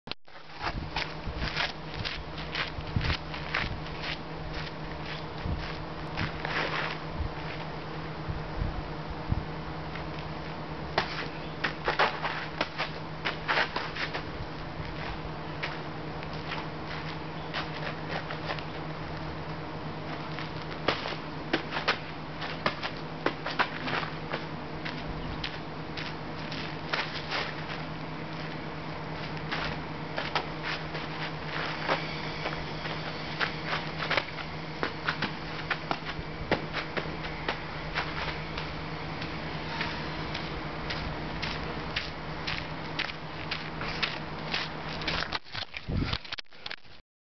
Off-Ice Combo Jumping
Here's a few 2/2 combination jumps.  Most of these looked fully rotated on slow motion replays.  Trying to fix my technique!  any pointers?